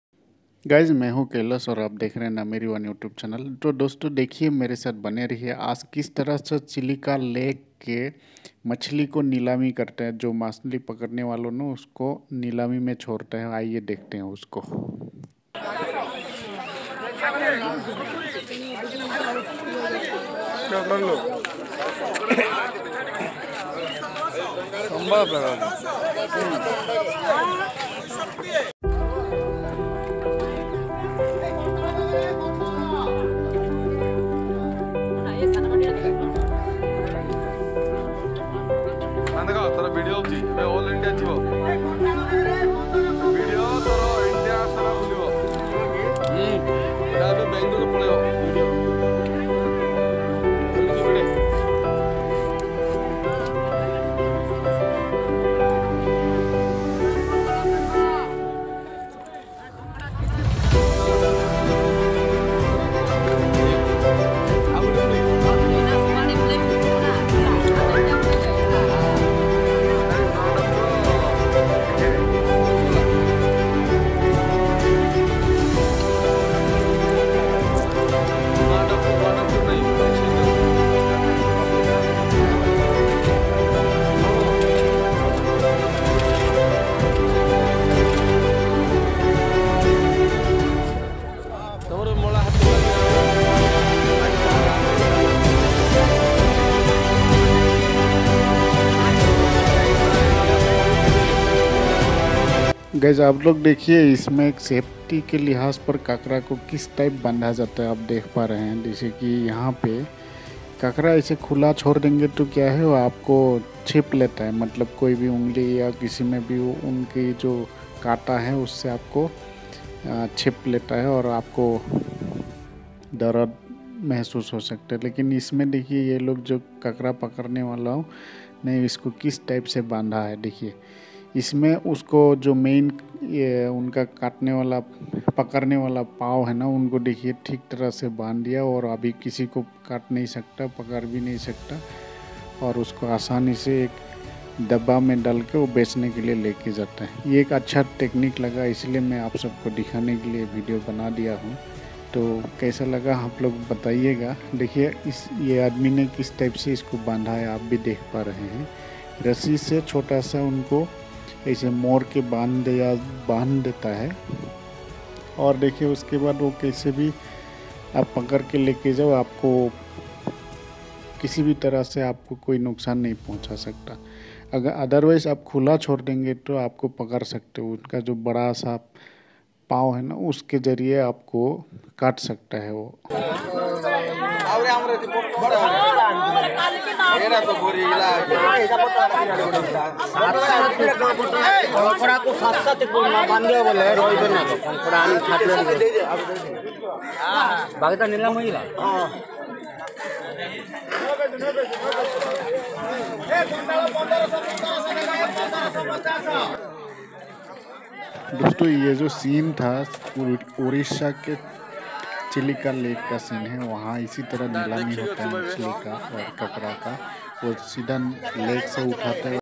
Chilika lake, chilika fish market
निवासियों और प्रवासी पक्षियों के अलावा भी, चिलिका वन्यजीव अभयारण्य में ब्लैकबक, चित्तीदार हिरण, गोल्डन जैकल्स, हाइना और कई और अधिक का भी घर है। जलीय वन्यजीवों की उपस्थिति के लिए समृद्ध रूप से प्रसिद्ध होने के कारण, चिल्का झील अभयारण्य में प्रॉन, डॉल्फिन, क्रैब, क्रस्टेशियंस और लिम्बलेस छिपकली भी रहता हैं।

चिलिका झील को लैगून भी कहा जाता है क्योंकि यह खारे पानी का एक उथला शरीर है ओर जो पानी के एक बड़े शरीर से अलग होता है। यह भारत का सबसे बड़ा तटीय लैगून है ओर जो लगभग 1100 किलोमीटर की दूरी तय करता है। यह ओडिशा राज्य में पुरी, गंजम और खुर्दा शहरों के पास स्थित है।

chilika lake
nameri one
chilika fish